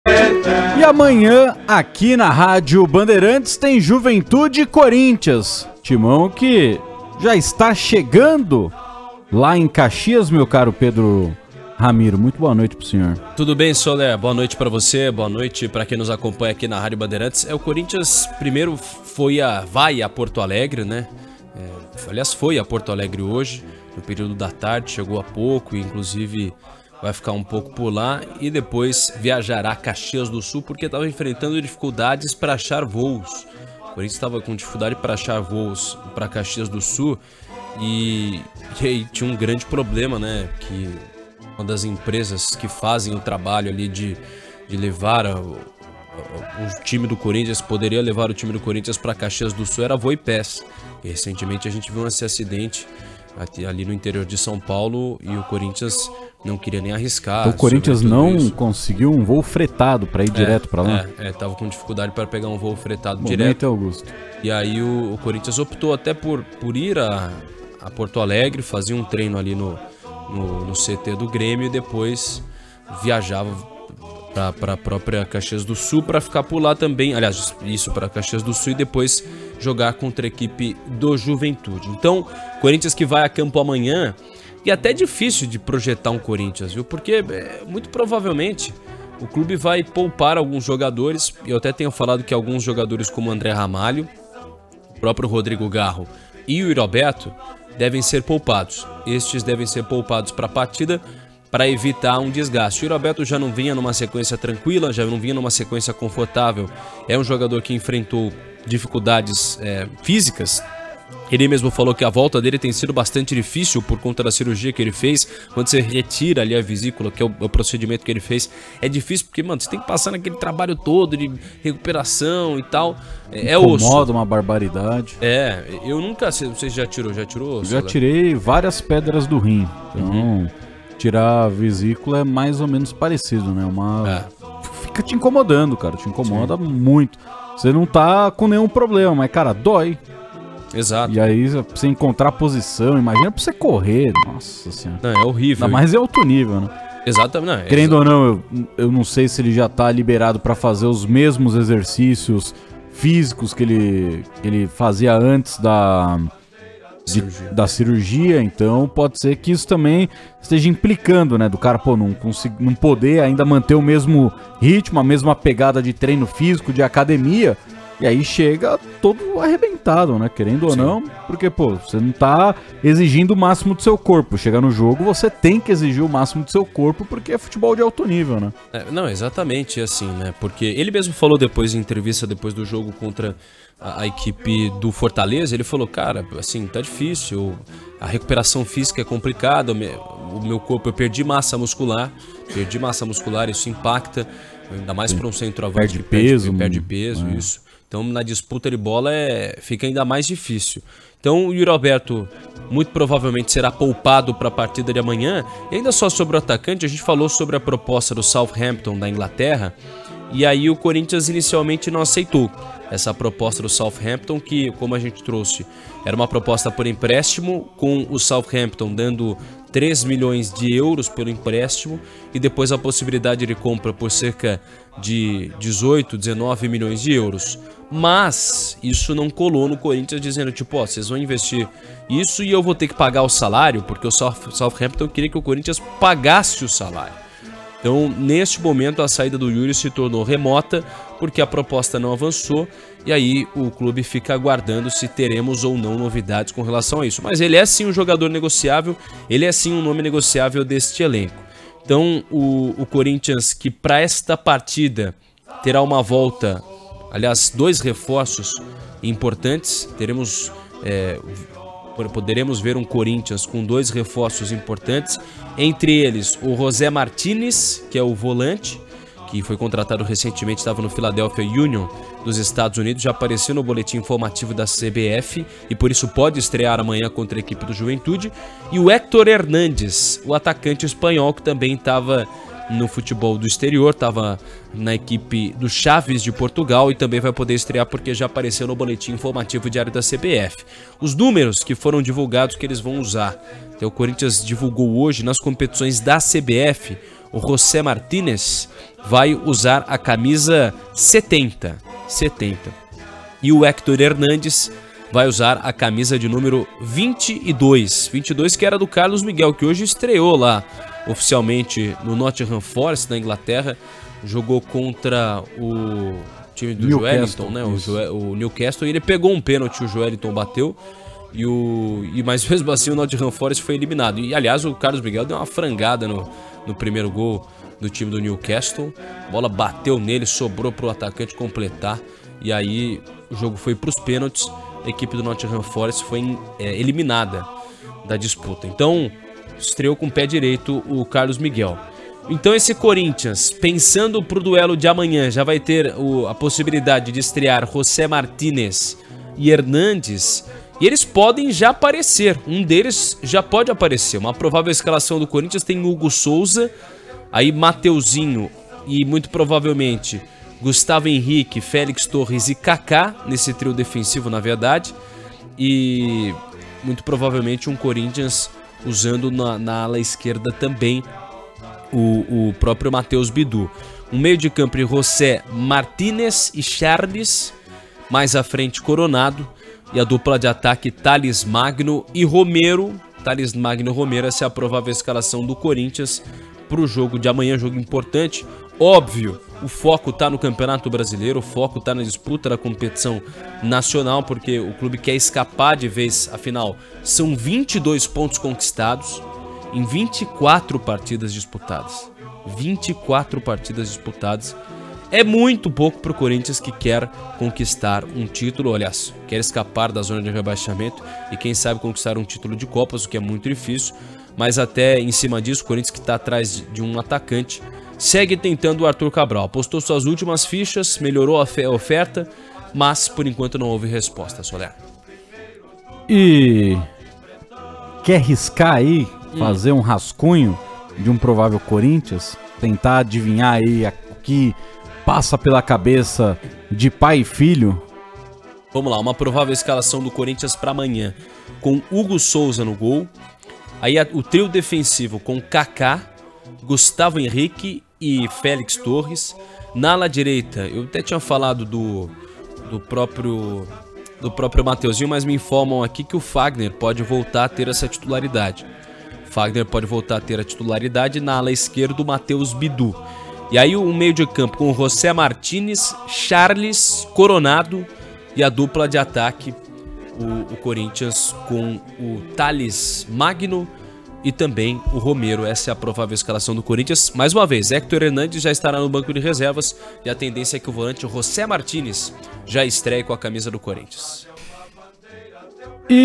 Corinthians enfrenta o Juventude com reforços | Os Donos da Bola
O repórter Pedro Ramiro traz informações sobre o confronto de quartas de final entre Corinthians e Juventude pela Copa do Brasil. Ramiro também fala sobre as ofertas por Yuri Alberto.